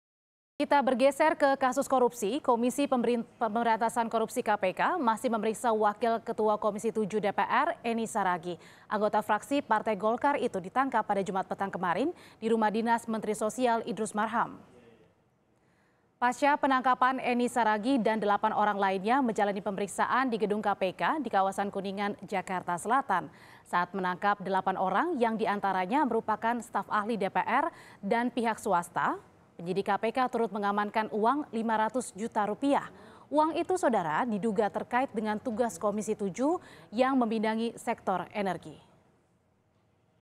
KPK Tangkap Eni Maulani Saragih, Wakil Ketua Komisi VII DPR
Penangkapan Eni diduga terkait tugas-tugasnya di komisi energi DPR. Pasca ditangkap pada Jumat (13/7) petang, Wakil Ketua Komisi Energi DPR Eni Maulani Saragih langsung menjalani pemeriksaan di Gedung Kpk di Kuningan, Jakarta Selatan.

Selain Eni, KPK menangkap delapan orang lainnyayang terdiri dari unsur staf ahli, sopir dan pihak swasta. Dalam penangkapan, penyidik KPK mengamankan barang bukti uang 500 juta rupiah.